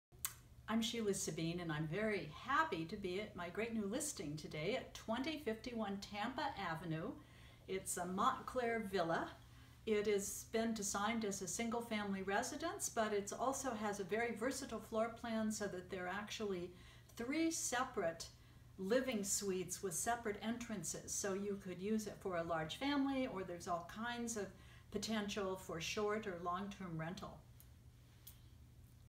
Sheila Sabine - New Listing - 2051 Tampa Ave., Oakland Part 1
Join me at my new Montclair listing